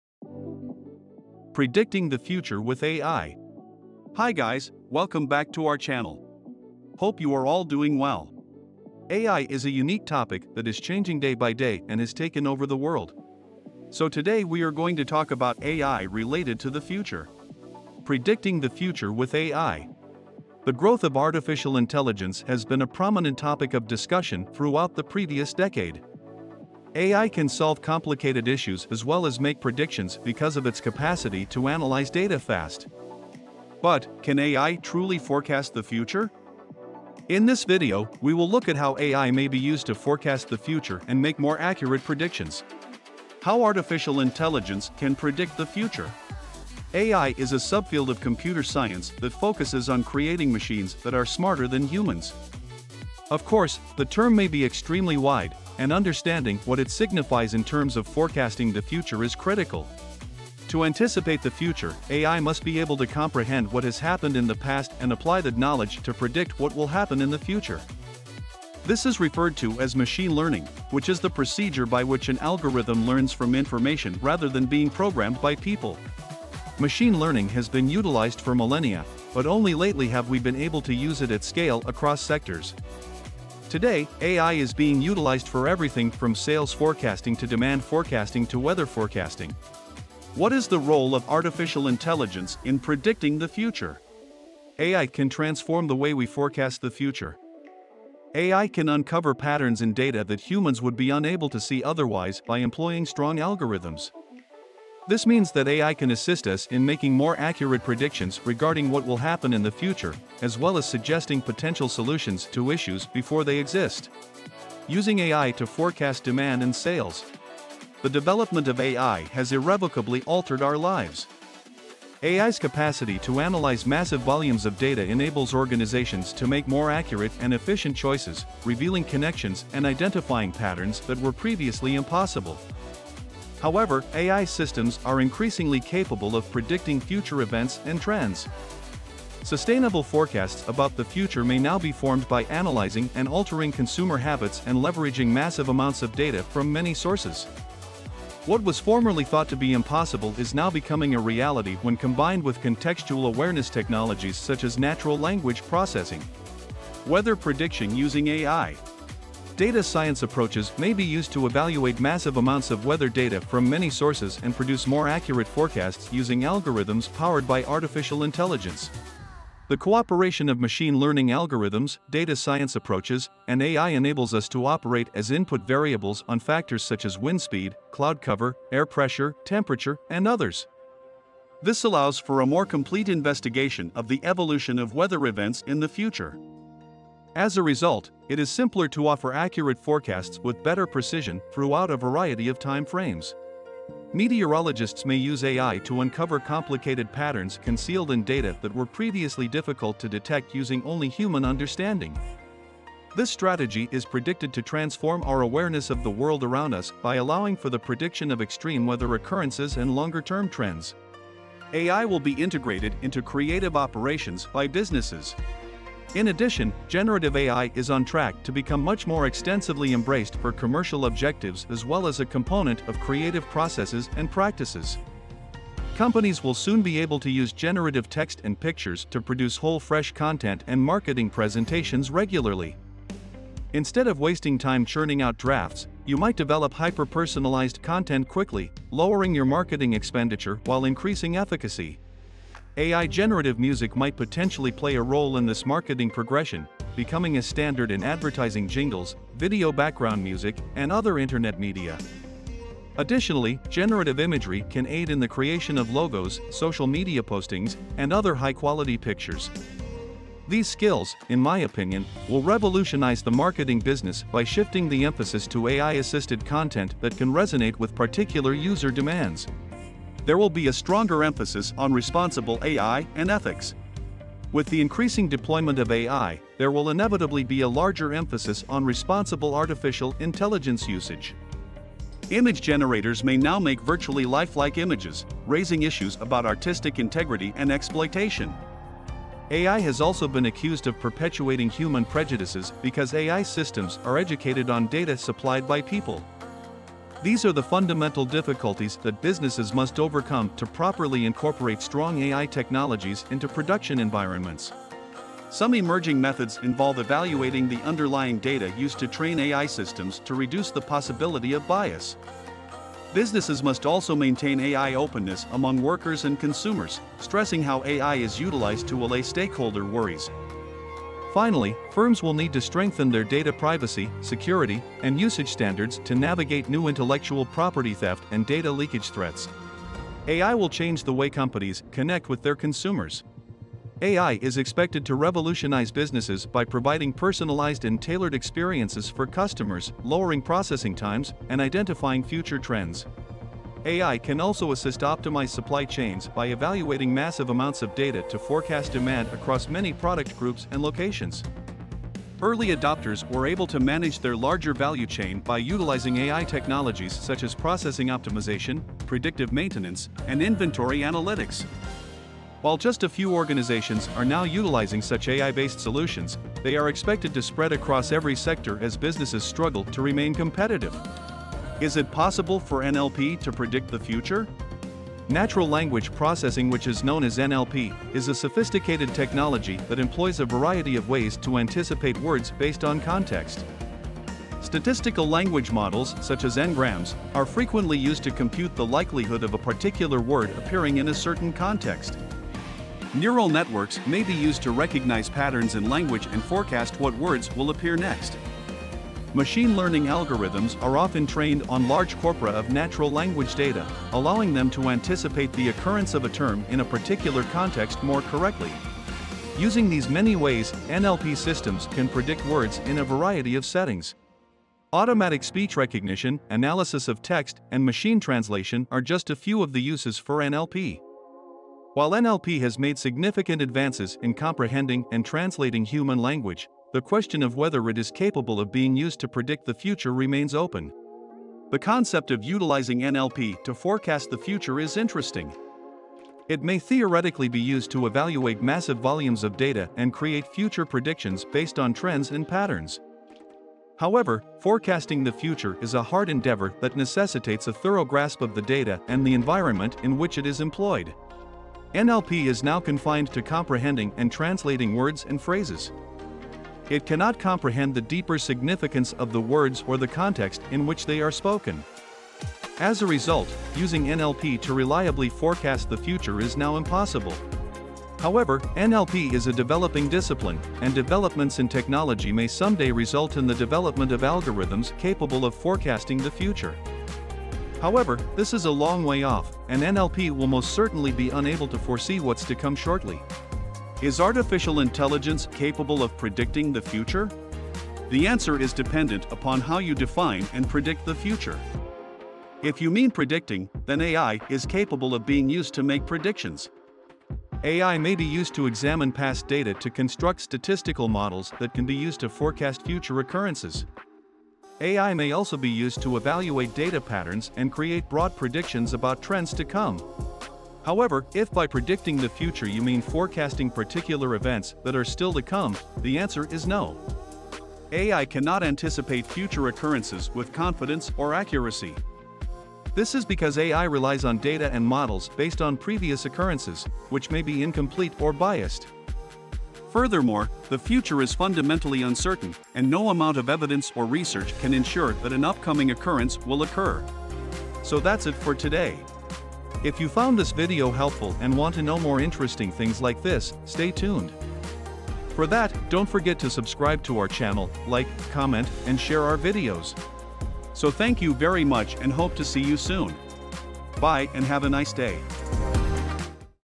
Predicting the future with AI
In this captivating video, we embark on an exhilarating journey into the boundless possibilities of Predictive Artificial Intelligence. 🌌 From forecasting trends to anticipating groundbreaking innovations, our AI experts will reveal the secrets behind how machines can foresee what lies ahead.

🔍 Curious about the power of predictive algorithms? 📈 Eager to understand how AI shapes our world tomorrow? Then you've come to the right place! Our expert team has meticulously crafted this video to demystify the science of prediction and its significant impact on various industries.

Get ready to be amazed as we discuss real-world applications, mind-boggling insights, and captivating case studies that showcase the astounding accuracy of AI predictions. 💡📚

Whether you're a tech enthusiast, a business visionary, or simply intrigued by the wonders of AI, this video promises to leave you enlightened and inspired!

Your journey into the future starts now. Let's explore the limitless world of AI together! 🌐🤝